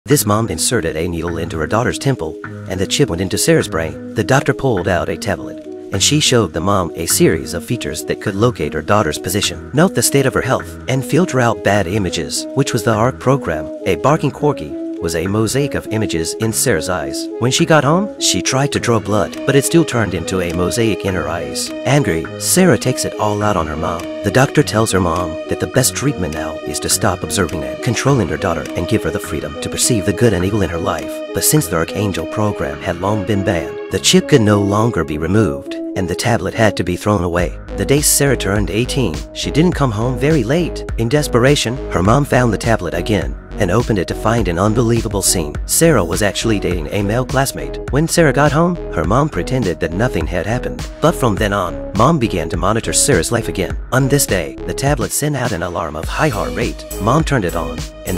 A mother puts a "monitor" in her daughter's head to keep an eye on her 1/2
A mother puts a "monitor" in her daughter's head to keep an eye on her.